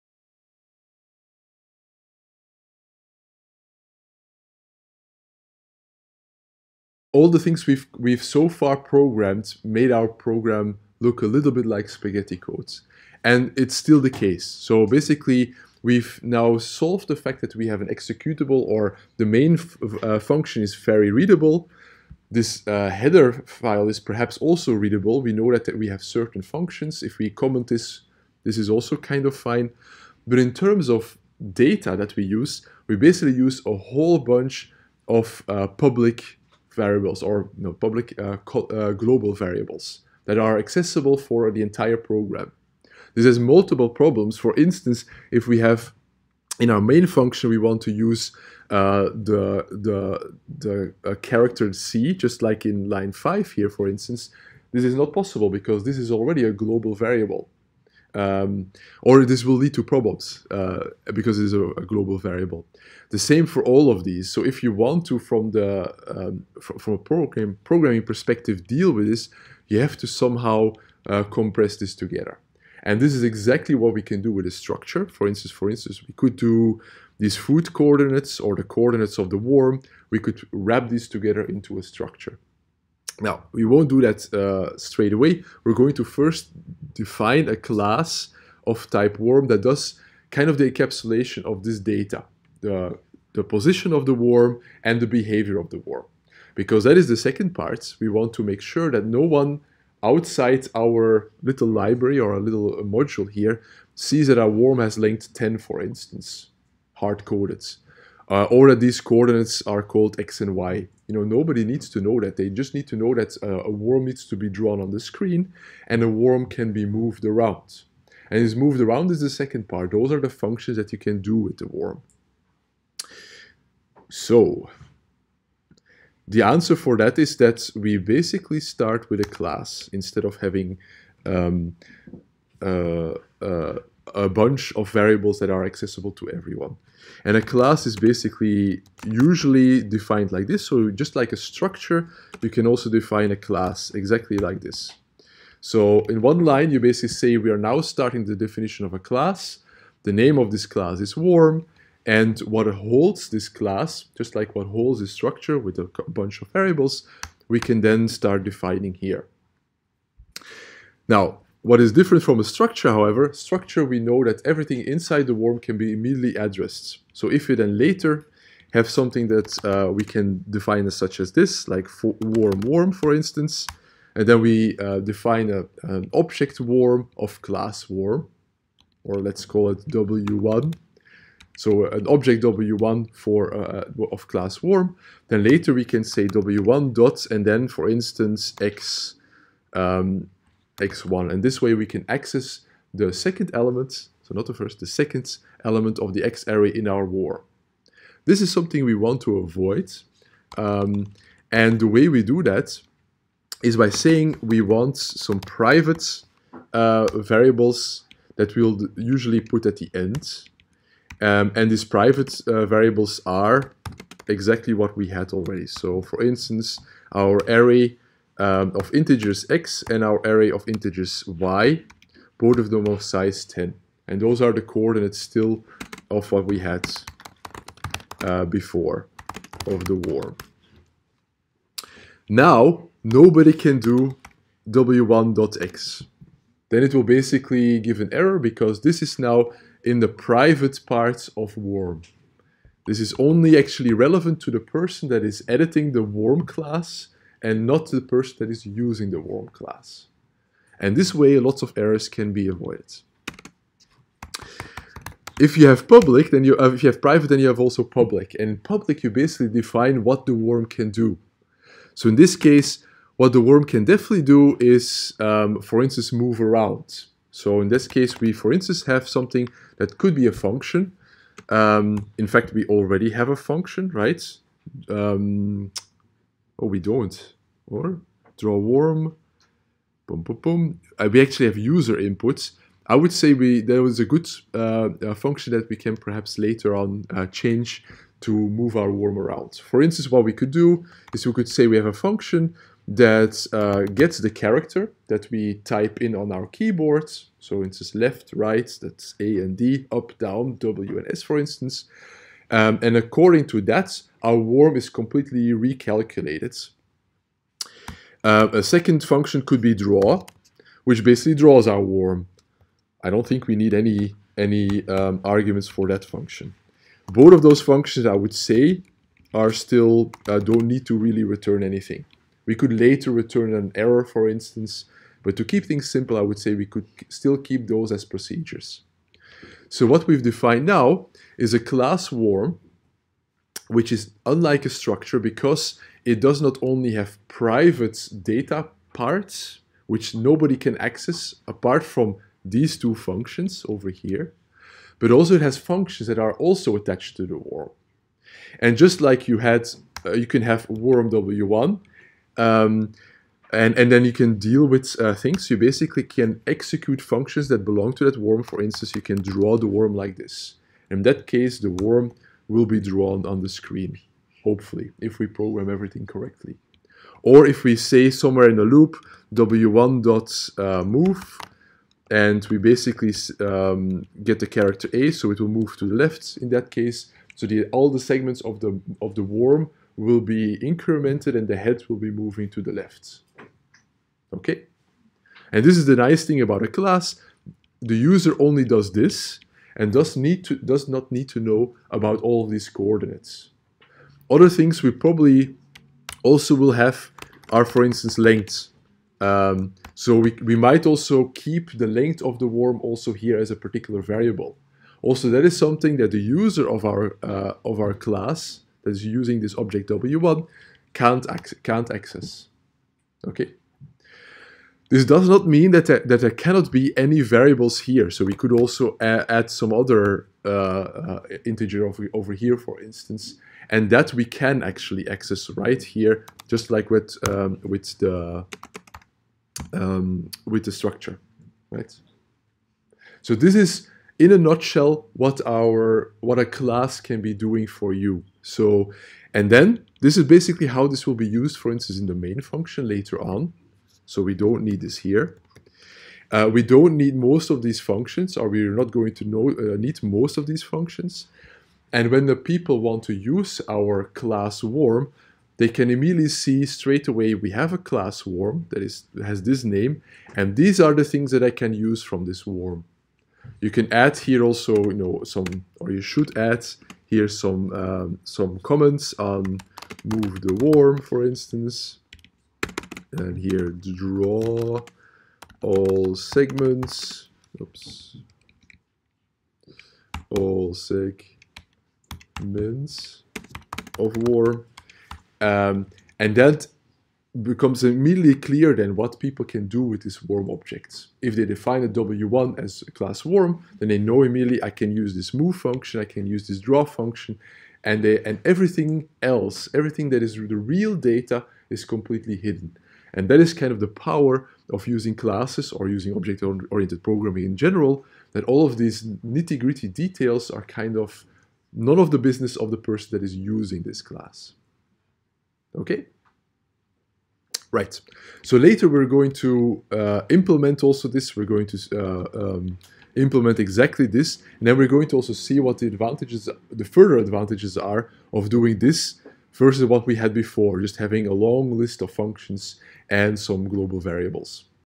i2p 09
Classes: How to group variables and functions together, using our worm game example --- part 1.
From: Introduction to Programming -- C and C++
by Prof. Dr. Kristof Van Laerhoven, University of Siegen